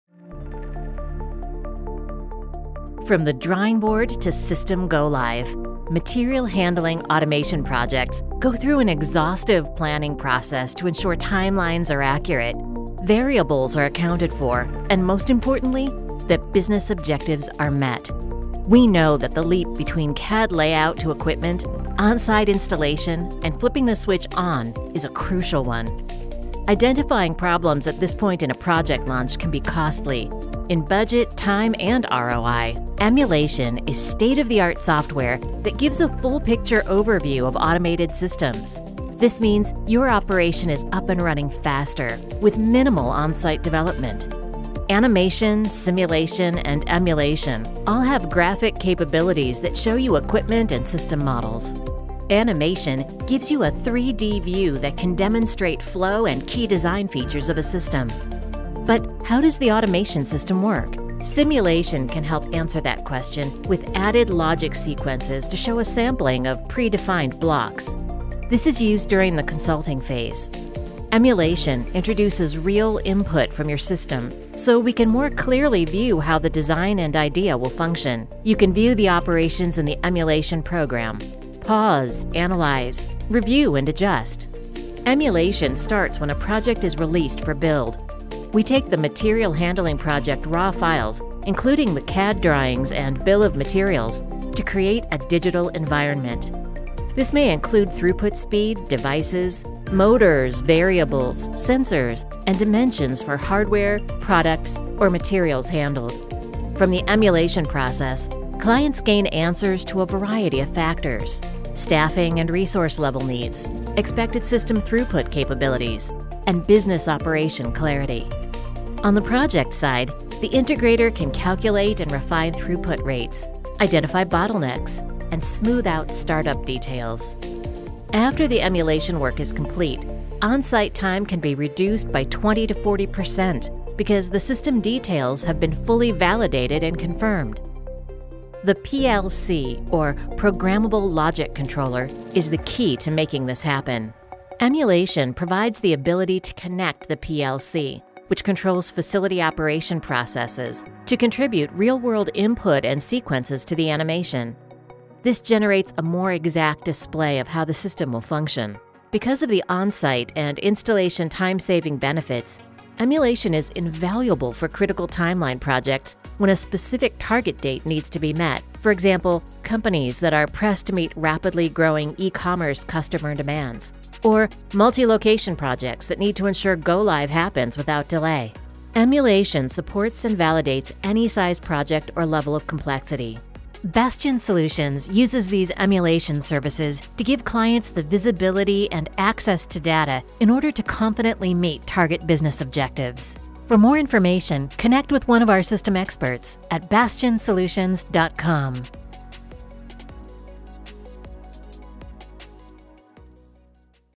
3D Software Maximize Warehouse Automated System ROI
This demo highlights the differences and opportunities for in-depth reviews of new automation technology and systems before they are delivered and installed. Learn more about how these simulations help us get your new operations up and running faster.

Using state of the art software during various phases of the project development process, animation, simulation, and emulation each bring different levels of graphical capabilities to demonstrate detailed models and operation movement. No matter the project size or level of complexity, these advanced 3D software tools give clients the visibility and access to data in order to confidently meet target business objectives.